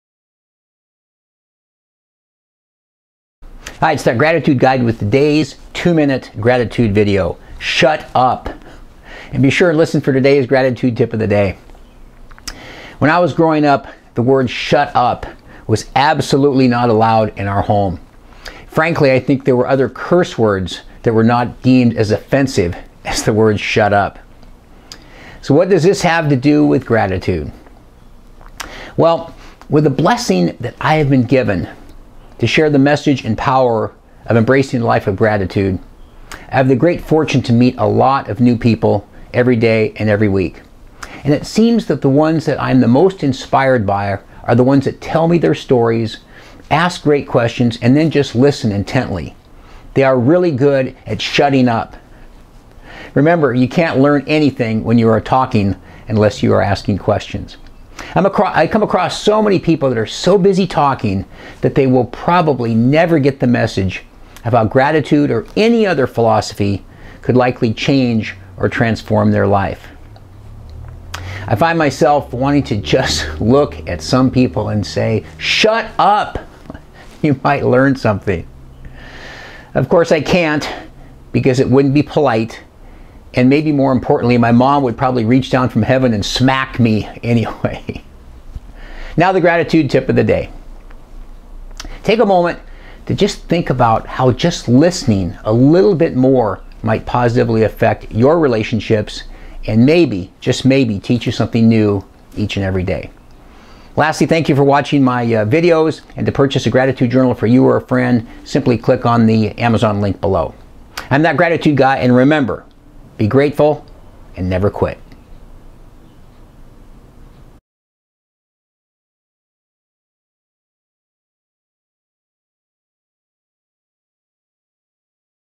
Shut Up!
Be sure and listen for today’s Gratitude Tip of the Day.

When I was growing up, the word Shut Up was absolutely not allowed in our home.  Frankly, I think there were other curse words that were not deemed as offensive as the words Shut Up.

So what does this have to do with gratitude?

Well, with the blessing that I have been given to share the message and power of embracing a life of gratitude, I have the great fortune to meet a lot of new people every day.  And it seems the ones that I am the most inspired by, are the ones that tell me their stories, ask questions, and then just listen intently.  They are good at shutting up.

Remember, you can’t learn anything when you are talking, unless you are asking questions.

I come across so many people that are so busy talking, that they will probably never get the message of how gratitude or any other philosophy could likely change or transform their life.

I find myself wanting to just look at some people and say Shut up!  You might learn something.

Of course, I can’t, because it wouldn’t be polite, and besides my Mom’ would likely reach down from heaven and smack me anyway!

Now the gratitude tip of the day:  Take a moment today to think about how just listening a little more might positively effect your relationships, and maybe teach you something new for the day.